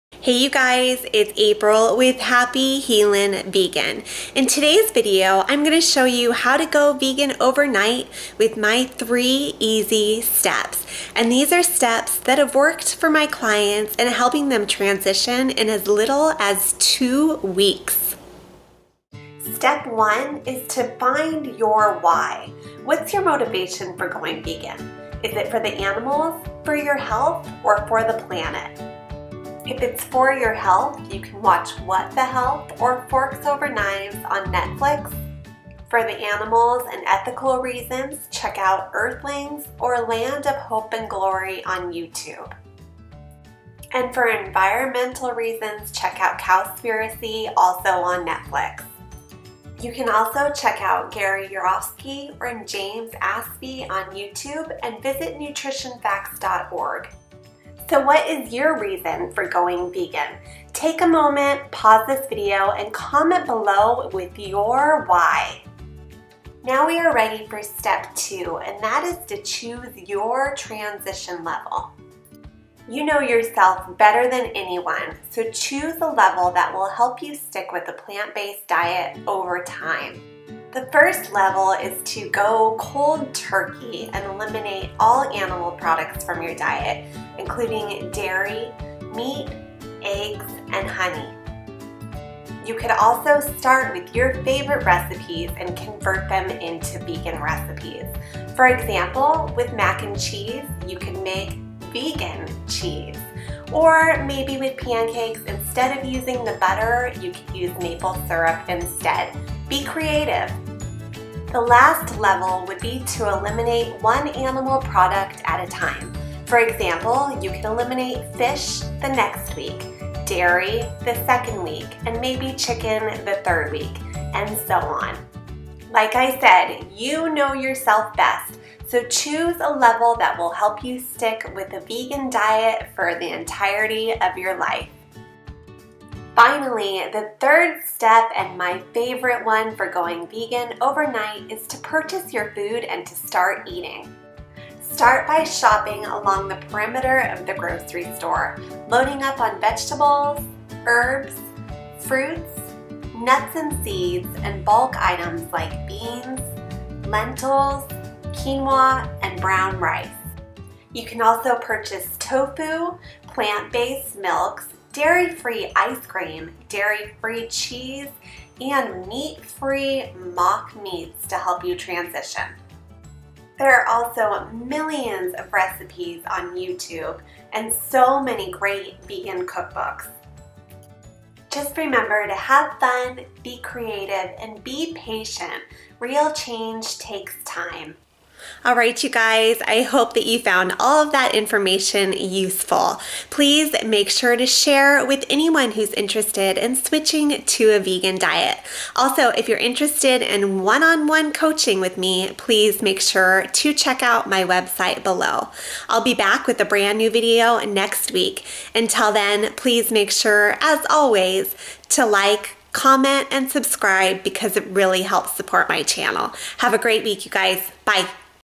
HOW TO GO VEGAN OVERNIGHT - MY 3 EASY STEPS
***Resources Mentioned During this Video***

*Land of Hope and Glory YOUTUBE

*Best Speech You Will Ever Hear - Gary Yourofsky YOUTUBE

*This Speech is Your WAKE UP CALL (James Aspey) YOUTUBE

*Oh She Glows Cookbook

*Oh She Glows Every Day

*The No Meat Athlete Cookbook

*Crazy Sexy Kitchen (Kris Carr)

***LET’S CONNECT!***

Over the last few weeks I have been lucky enough to come across people who are interested in switching to a vegan diet but just aren’t sure where to start.  Although, going vegan overnight may not work for everyone, I wanted to put together some resources for those who are interested in giving this vegan diet a try.

In this video I will recommend motivational documentaries and Youtube videos, help you figure out your “level” of transitioning and take you around the grocery store.

Thank you all for your support in growing my channel.